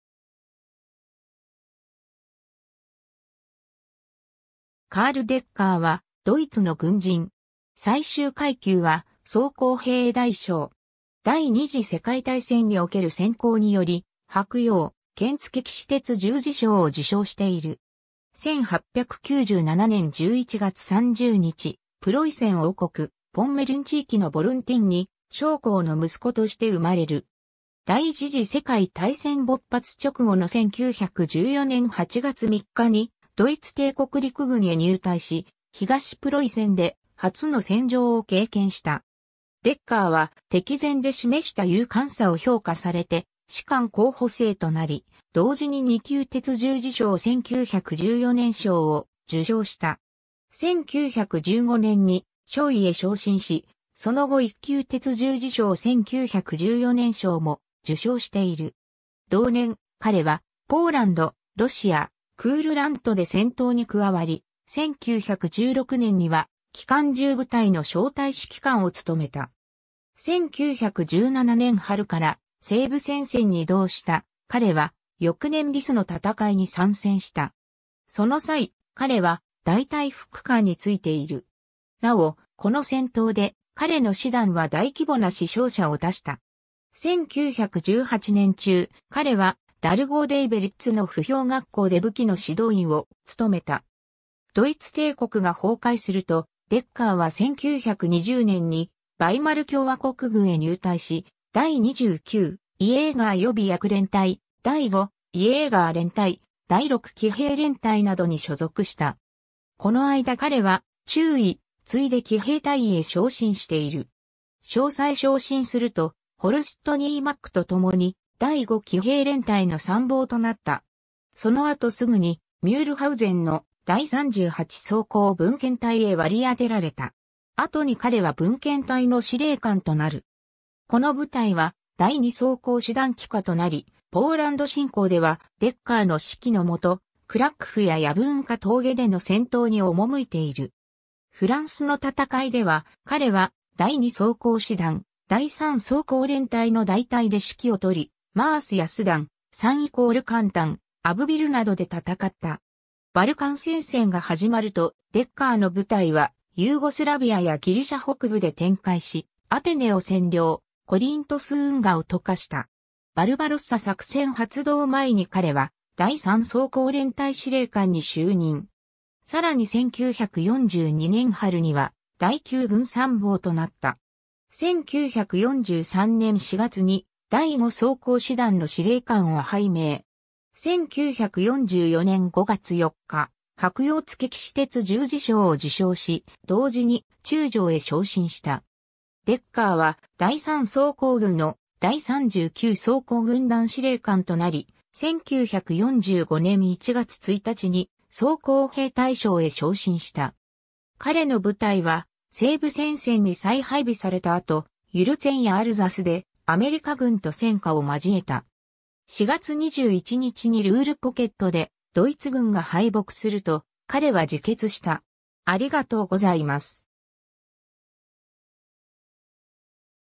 カール・デッカー
カール・デッカー（ドイツ語: Karl Decker、1897年11月30日 - 1945年4月21日）は、ドイツの軍人。
最終階級は装甲兵大将。
第二次世界大戦における戦功により柏葉・剣付騎士鉄十字章を受章している。
 1897年11月30日、プロイセン王国・ポンメルン地域のボルンティンに将校の息子として生まれる。
第一次世界大戦勃発直後の1914年8月3日にドイツ帝国陸軍へ入隊し、東プロイセンで初の戦場を経験した。
デッカーは敵前で示した勇敢さを評価されて士官候補生となり、同時に二級鉄十字章1914年章を受章した。
1915年に少尉へ昇進し、その後一級鉄十字章1914年章も受章している。
同年、彼はポーランド、ロシア、クールラント（現在のラトビア）で戦闘に加わり、1916年には機関銃部隊の小隊指揮官（ツークフューラー）を務めた。
1917年春から西部戦線に移動した彼は、翌年リスの戦いに参戦した。
その際、彼は大隊副官に就いている。
なお、この戦闘で彼の師団は大規模な死傷者を出した。
1918年中、彼はダルゴウ＝デーベリッツの歩兵学校で武器の指導員を務めた。
 ドイツ帝国が崩壊すると、デッカーは1920年にヴァイマル共和国軍へ入隊し、第29「イェーガー」予備役連隊、第5「イェーガー」連隊、第6騎兵連隊などに所属した。
この間彼は中尉、次いで騎兵大尉へ昇進している。
少佐へ昇進すると、ホルスト・ニーマックとともに第5騎兵連隊の参謀となった。
その後すぐにミュールハウゼンの第38装甲分遣隊へ割り当てられた。
後に彼は分遣隊の司令官となる。
 この部隊は第2装甲師団麾下となり、ポーランド侵攻ではデッカーの指揮の下クラクフやヤブウォンカ峠での戦闘に赴いている。
フランスの戦いでは、彼は第2装甲師団・第3装甲連隊の大隊で指揮を執り、マースやスダン、サン＝カンタン、アブヴィルなどで戦った。
 バルカン戦線 (第二次世界大戦)が始まると、デッカーの部隊はユーゴスラビアやギリシャ北部で展開しアテネを占領、コリントス運河を渡河した。
バルバロッサ作戦発動前に彼は第3装甲連隊司令官に就任。
さらに1942年春には第9軍参謀となった。
1943年4月に第5装甲師団の司令官を拝命。
1944年5月4日、柏葉付騎士鉄十字章を受章し、同時に中将へ昇進した。
 デッカーは第3装甲軍の第39装甲軍団司令官となり、1945年1月1日に装甲兵大将へ昇進した。
彼の部隊は西部戦線に再配備された後、ユルツェンやアルザスでアメリカ軍と戦火を交えた。
4月21日にルール・ポケットでドイツ軍が敗北すると、彼は自決した。